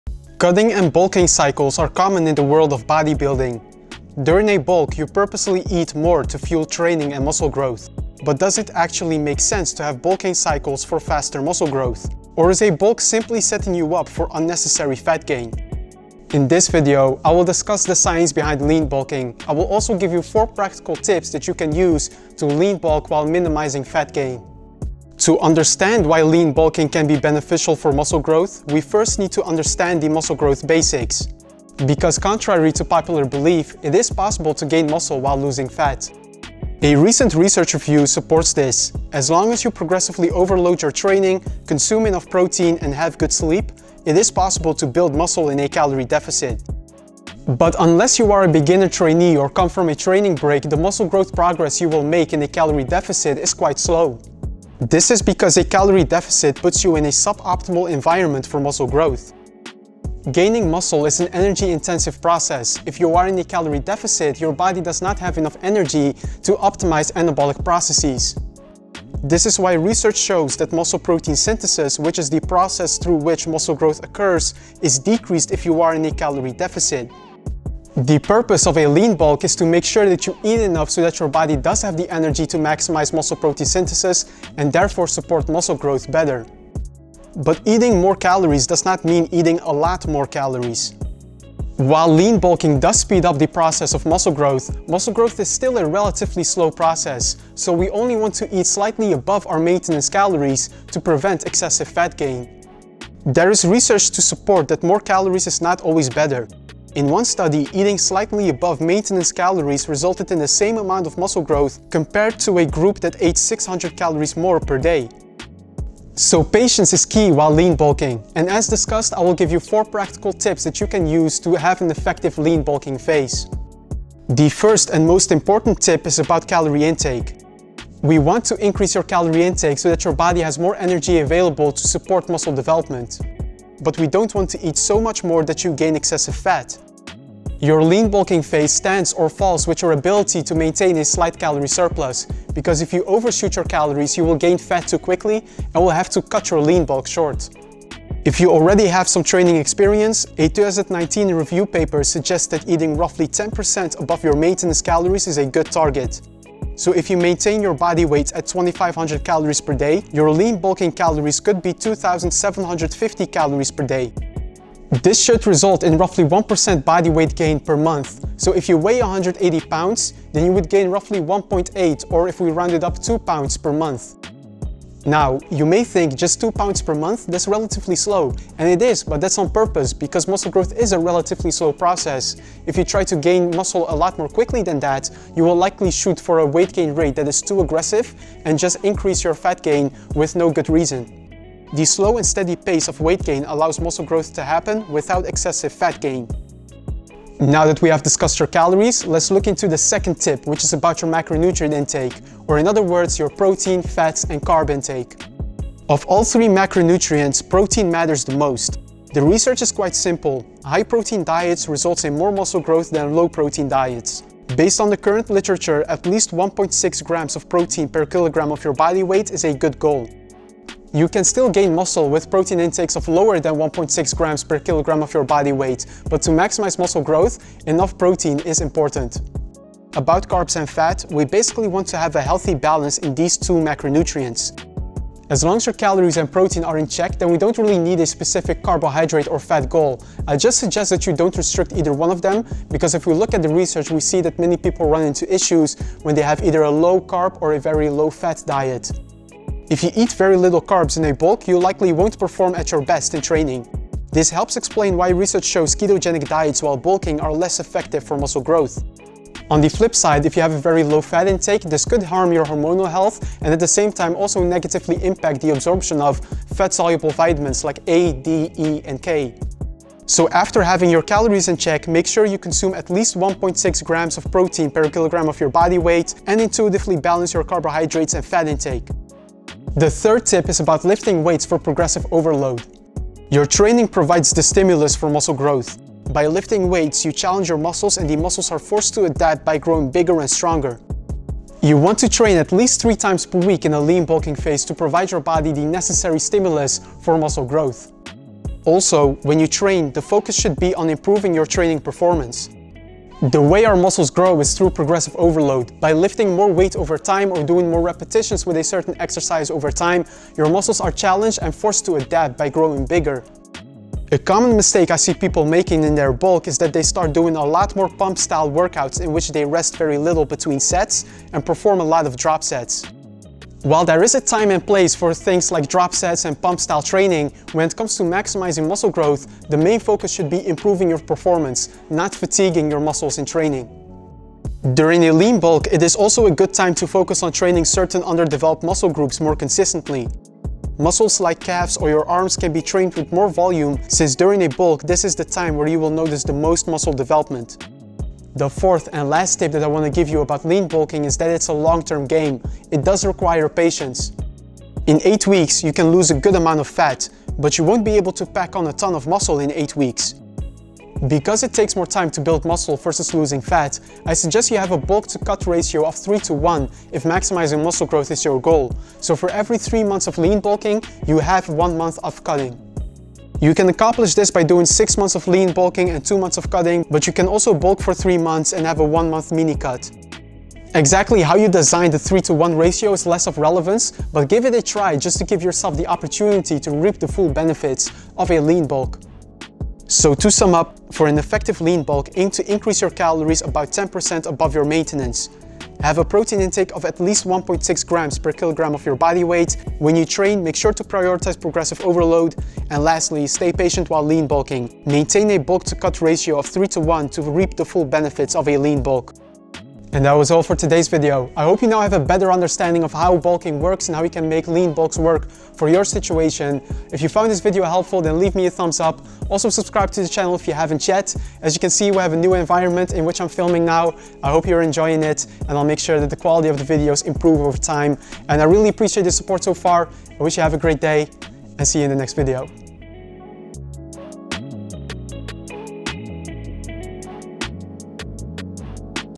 Does Bulking Actually Work? | 4 Steps For A Lean Bulk
Wondering how you can have an effective bulking phase? Then this video is for you! I will discuss how you can have a lean bulk without getting fat.

Cutting and bulking cycles are common in the world of bodybuilding. During a bulk, you purposely eat more to fuel training and muscle growth. But does it actually make sense to have bulking cycles if you want faster muscle growth? Or is a bulk simply setting you up for unnecessary fat gain?

In this video, I will discuss the science behind bulking phases and will also cover 4 tips on how you can bulk without gaining excessive fat. This is also known as lean bulking. To understand why lean bulking can be beneficial for muscle growth, we first need to understand the muscle growth basics. Because contrary to popular belief, it is possible to gain muscle while losing fat. As long as you progressively overload your training, consume enough protein, and have good sleep, it is possible to build muscle in a calorie deficit.

But unless you are a complete beginner trainee or come from a training break, the muscle growth progress you will make while in a calorie deficit is quite slow. This is because a calorie deficit puts you in a suboptimal environment for muscle growth. Gaining muscle is a process that requires much energy. In a calorie deficit, your body does not have the required energy to optimize muscle tissue growth. The purpose of a bulk is to eat more so that your body does have the energy required to maximize muscle protein synthesis and, therefore, muscle growth.

But eating more does not mean eat a lot more. While bulking speeds up the process of gaining muscle, muscle growth is still a relatively slow process. Especially if you already have some training experience. So we only want to eat slightly above our maintenance calories to prevent excessive fat gain.

The 4 tips I discuss in this video for an effective lean bulk are about:

1. Calorie-Intake
2. Macronutrient Intake
3. Strength Training
4. Bulking/Cutting ratios